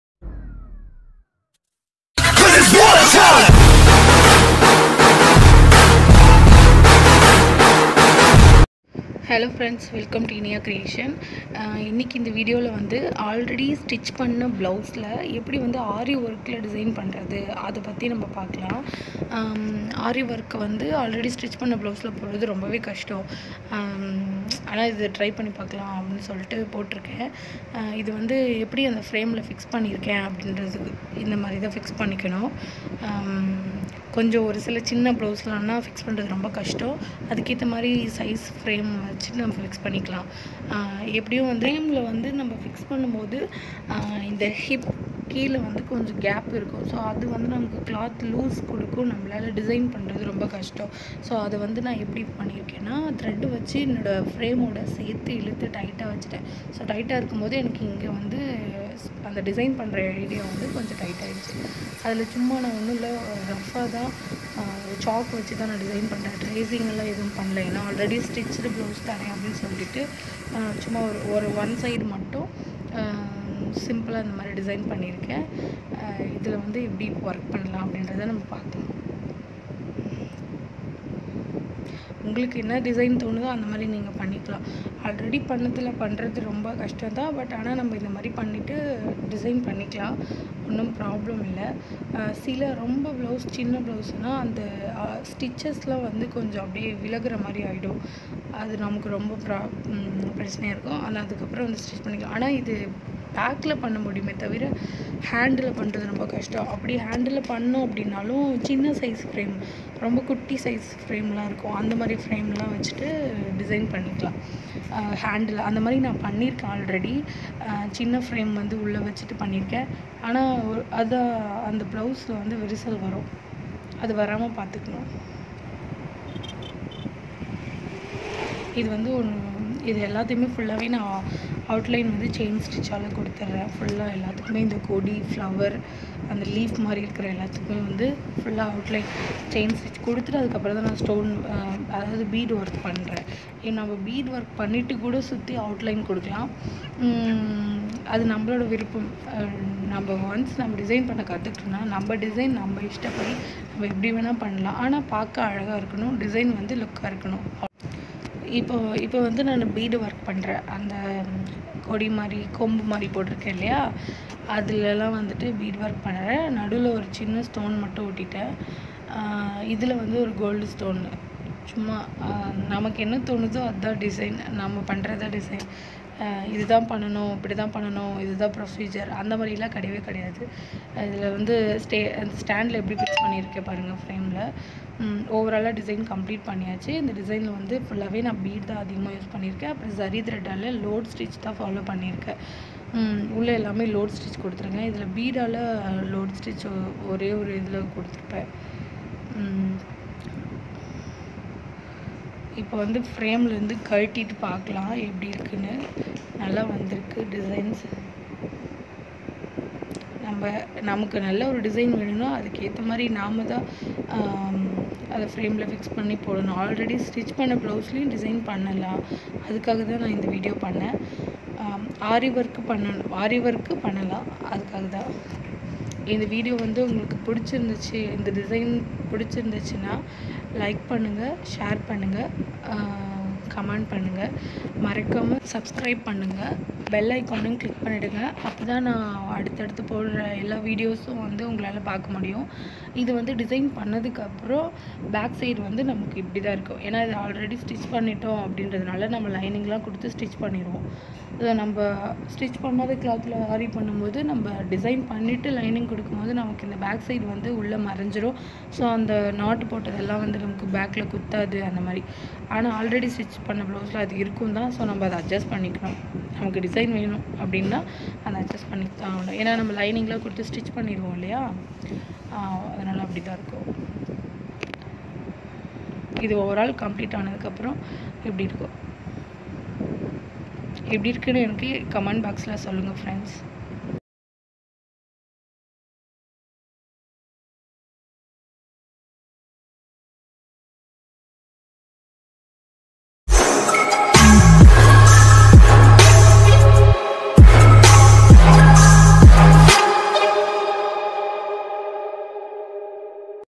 Aari work on stitched blouse||How to fix stitched blouse on frame|simple design
Hi friends
 welcome to Iniya creation's

In this video i shared how to design aari work on already stitched blouse and how to fix that blouse on frame for designing